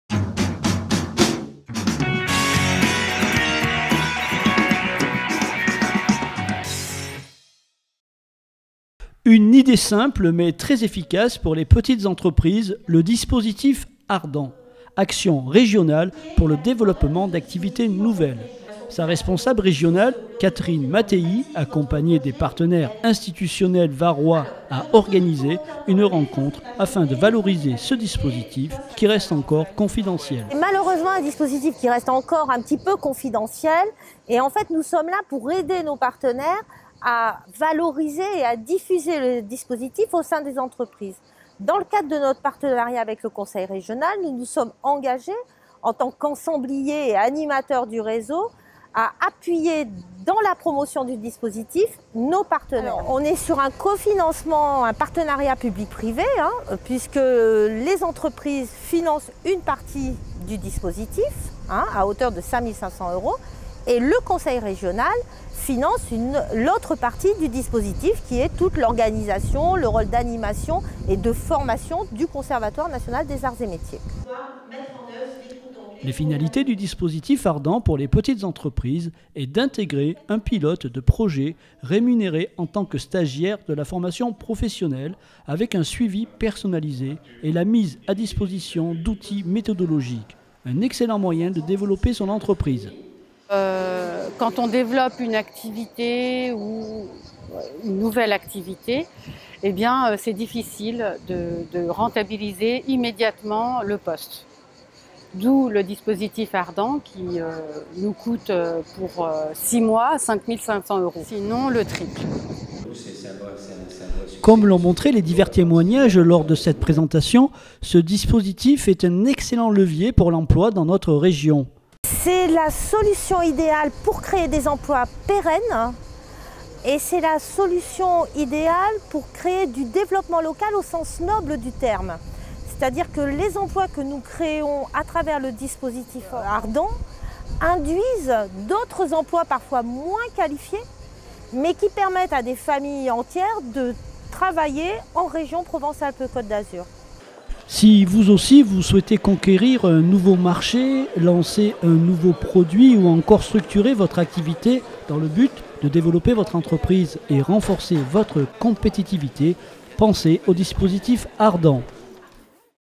Le dispositif Ardan en PACA
ARDAN, Action Régionale pour le Développement d'Activités Nouvelles, est un dispositif qui permet aux petites entreprises, entreprises Artisanales et associations de concrétiser leur projet en intégrant un collaborateur rémunéré en tant que stagiaire de la formation professionnelle continue